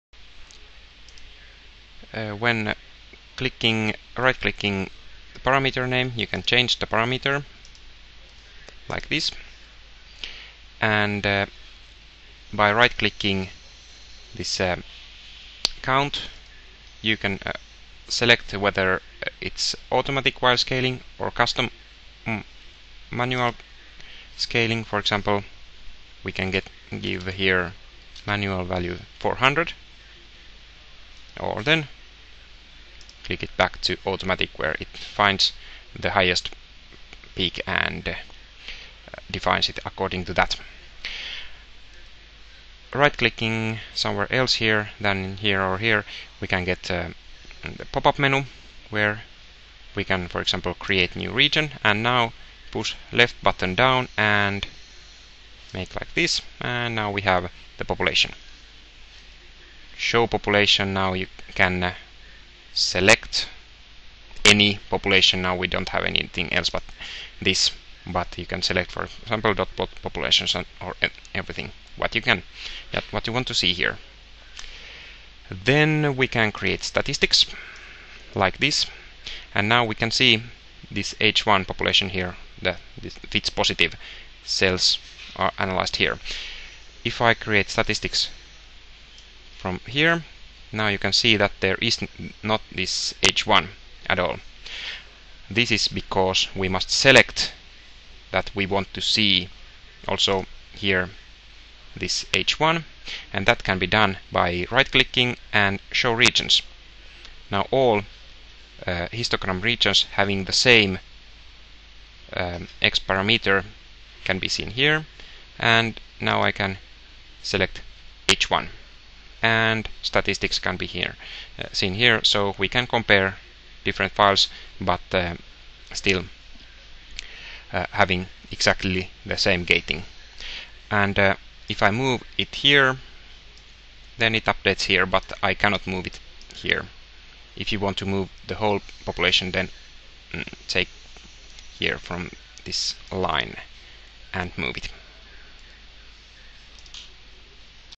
02. How to work with histograms
How to change X parameters and Y scaling. Choose which regions to visualize and create new populations. Create histogram statistics.